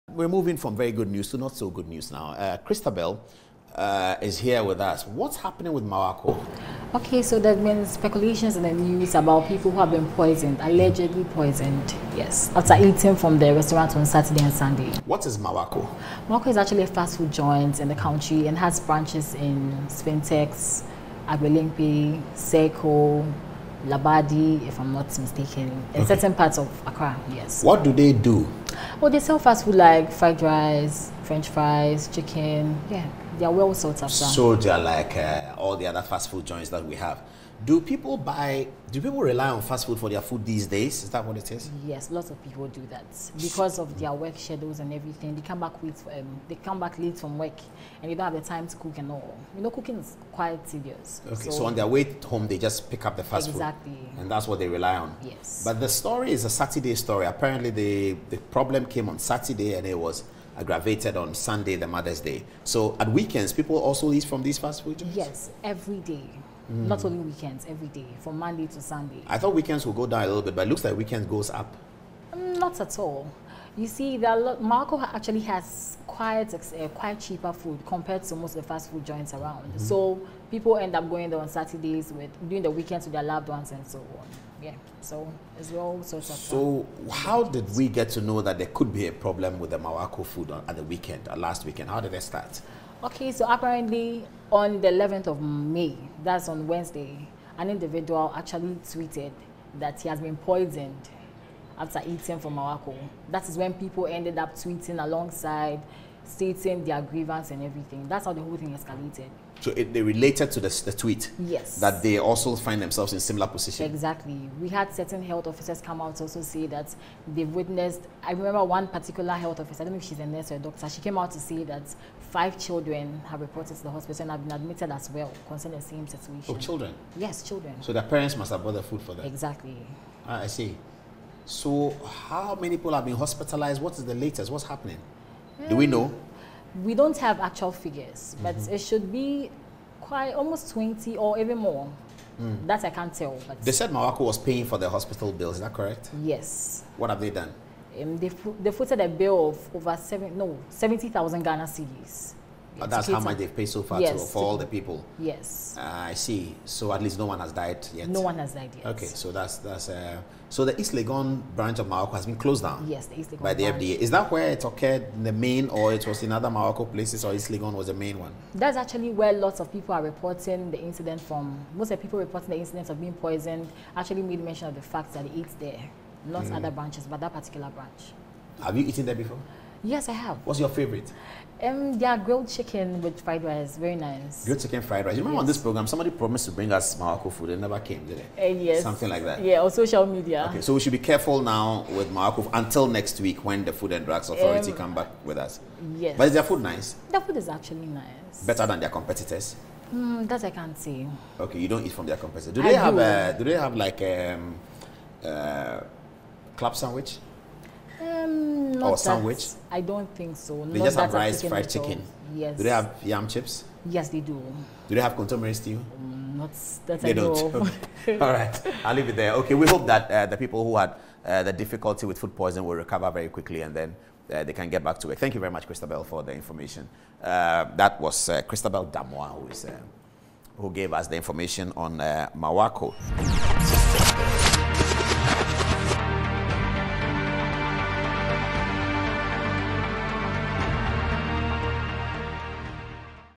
Marwako alleged food poisoning: FDA conducts investigations into customers’ complaints.
Our young lady Christabel Damoah had been following the Marwako incident of alleged food poisoning. She brought us the latest, gratifying to note that the FDA were on the beat. See video and stay safe this weekend, stay away from food poisoning... Video... 4mins

 @Paul Adom-Otchere